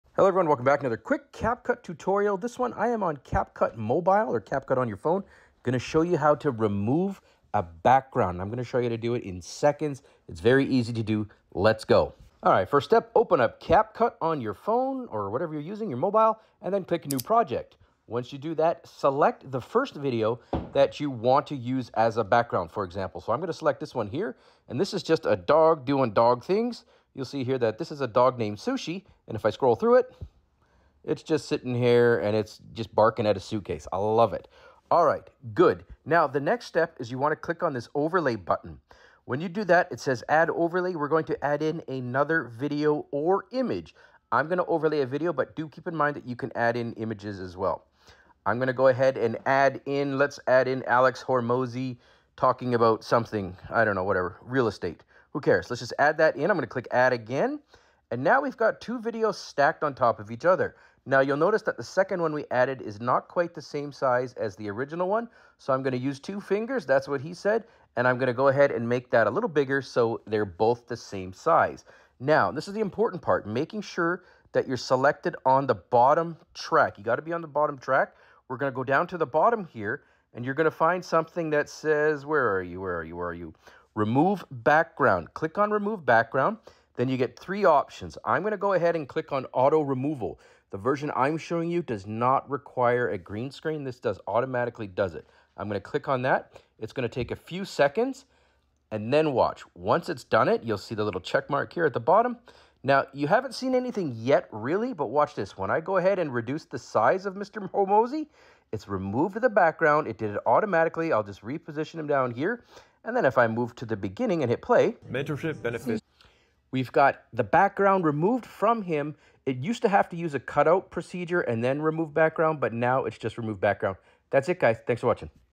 Capcut - Remove Background With 1 Click - Mobile Version (2024)
Capcut Mobile - Remove any Video or Photo Background - 1 Click!

In today's tutorial, we're diving into the simplicity of background removal using CapCut on your mobile device. If you've ever struggled with complex editing software or tedious manual cut-outs, this guide is a game-changer. Let's dive in!

📱 Starting Point [00:00:00]:

Introduction to the CapCut mobile app.
Overview of the quick and easy background removal process.
🐕 Select Your Footage [00:00:15]:

Initiating a new project in CapCut.
Choosing a video clip - featuring "Sushi" the dog, engaging in playful antics.
🎞️ Overlay Addition [00:00:34]:

How to use the overlay feature to add additional media.
Demonstrating with a video clip of Alex Hermosa discussing real estate.
🔄 Adjusting Overlays [00:00:55]:

Resizing and stacking videos to match dimensions.
🚫 Background Removal Magic [00:01:09]:

Navigating the CapCut interface to the background removal feature.
Selecting the auto removal option for effortless editing.
✨ Final Touches [00:01:28]:

Witnessing the automatic background removal in action.
Adjusting the subject's position within the frame.
🔚 Wrap-Up [00:01:44]:

Showcasing the final product with a background-free video.
Highlighting the efficiency of the new CapCut feature.
Thank you for tuning into our quick CapCut background removal tutorial. We hope this guide makes your mobile video editing smoother and more enjoyable. Happy editing!